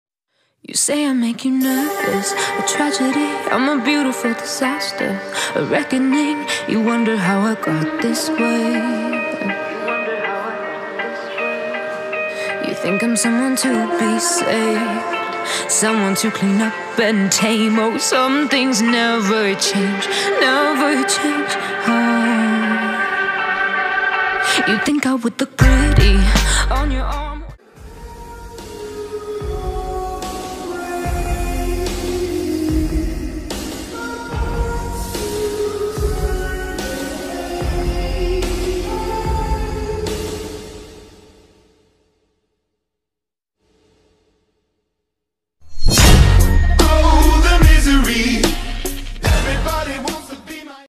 pov: temanmu bilang ada kebakaran di sekolah mu, tetapi kmu tdk percaya, dan..
hai?:v, ib: @//kiran

Maaf klo alur nya gaje / jelek yh
abaikan hastag ! :D